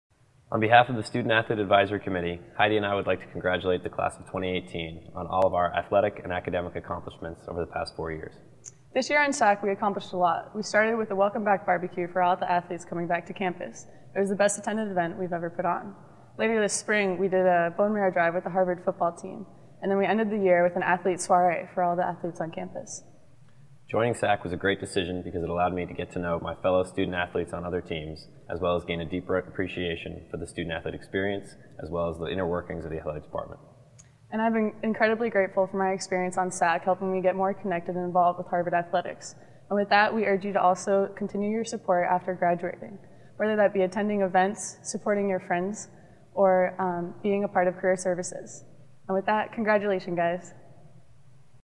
Congratulatory Message from the Student-Athlete Advisory Committee
Student-Athlete Advisory Committee co-chairs Heidi Nocka '18 and Noah Zavolas '18 share a congratulatory message for their classmates at the 2018 Senior Letterwinners' Dinner